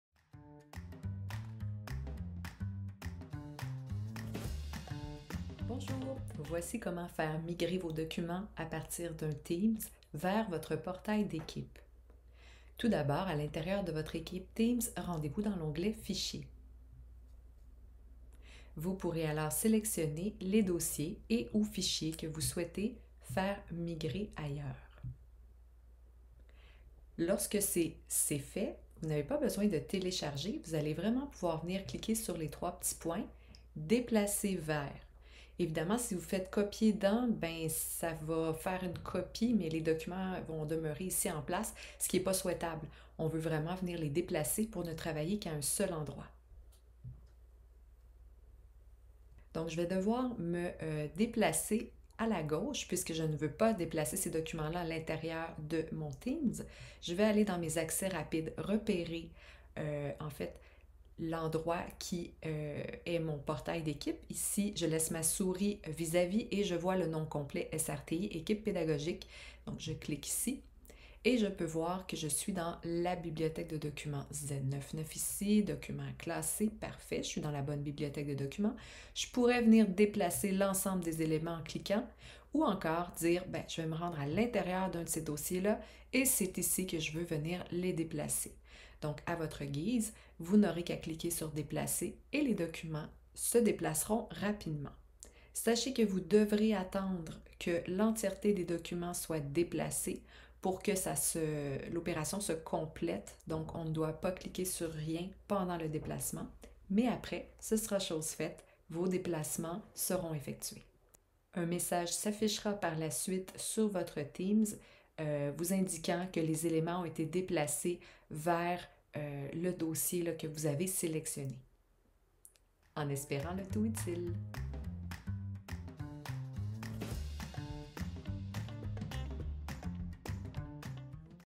Déplacer des documents de Teams vers portail d'équipe
Voici comment déplacer vos documents présentement hébergés dans un Teams vers votre portail d'équipe SharePoint.